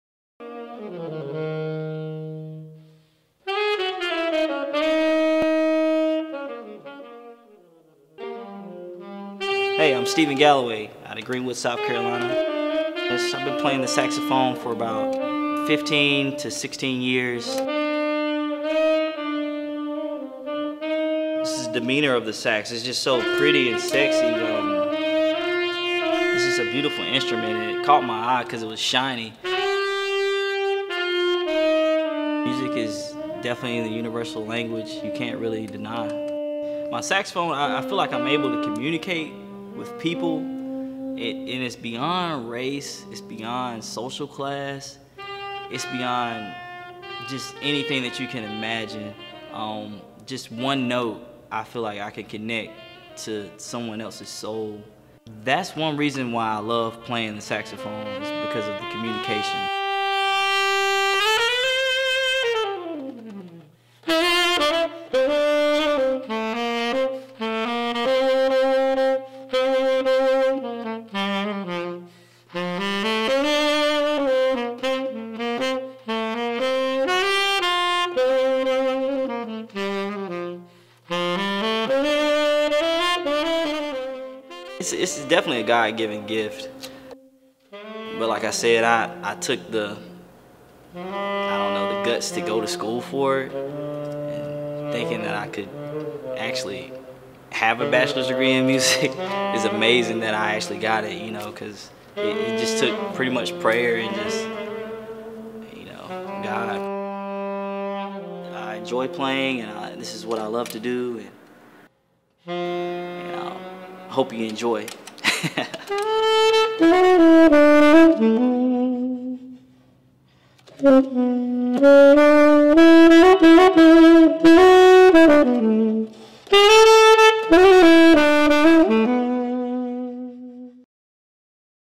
Portrait of Saxaphon Player - Steve Galloway
This is a portrait piece on saxophonist Steve Galloway from Greenwood, SC